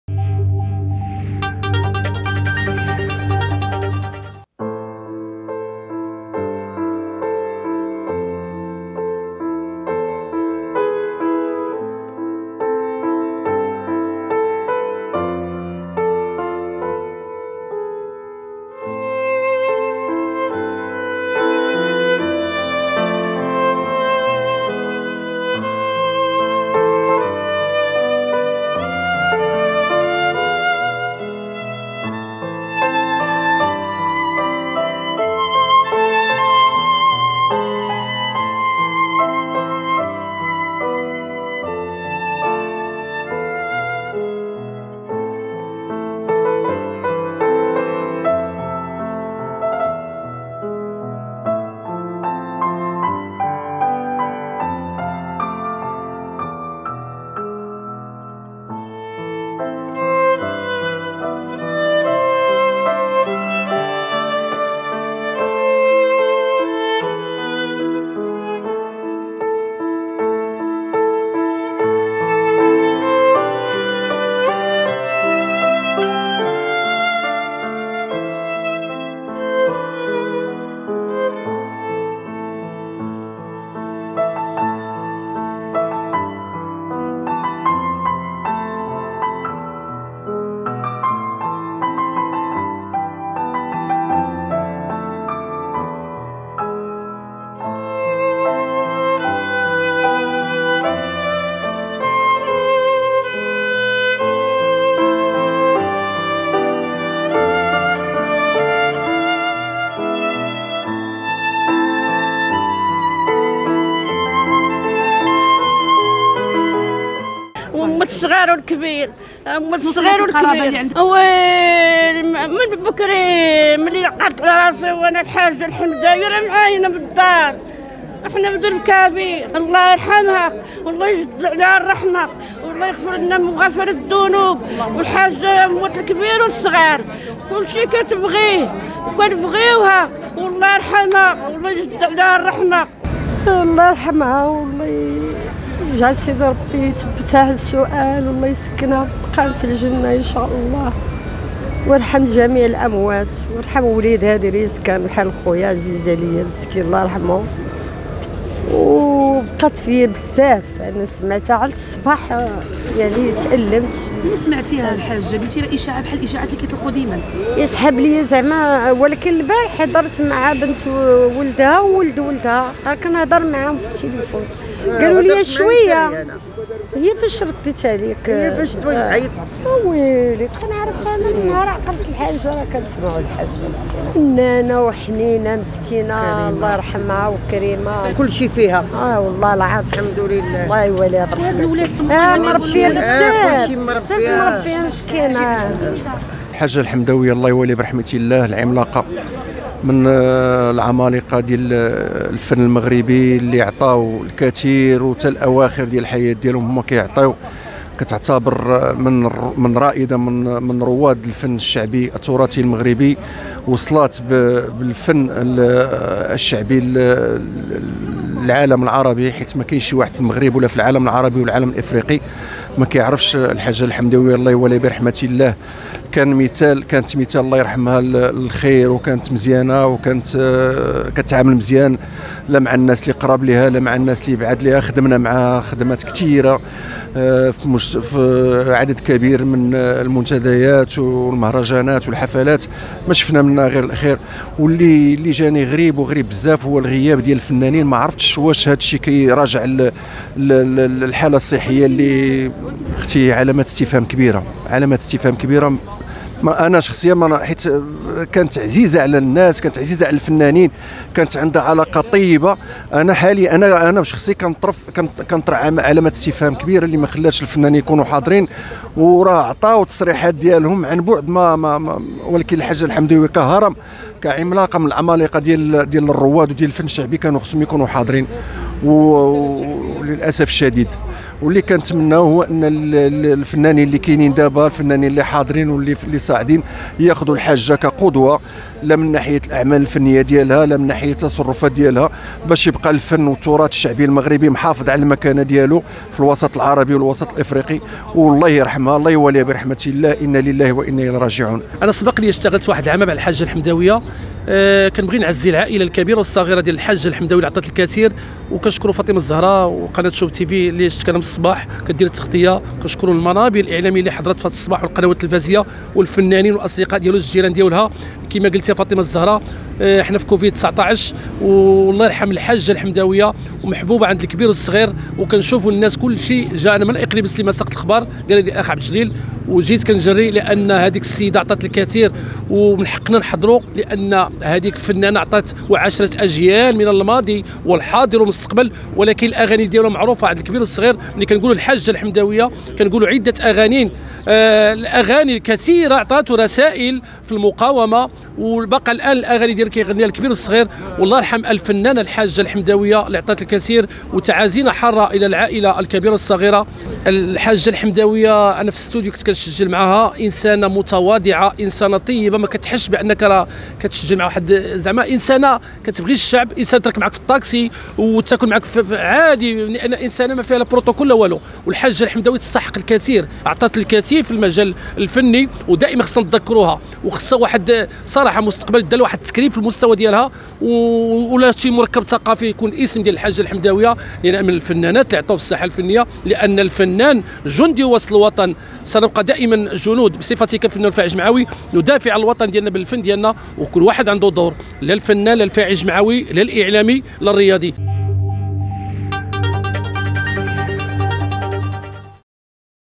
جنازة مهيبة لأيقونة العيطة "الحاجة الحمداوية" وشهادات بالدموع لحظة دفنها بمقبرة الشهداء
توفيت الفنانة الشعبية الحاجة الحمداوية ،صباح اليوم الاثنين بأحد مستشفيات الرباط، بعد معاناة طويلة مع المرض، حسب ما علم لدى عائلتها.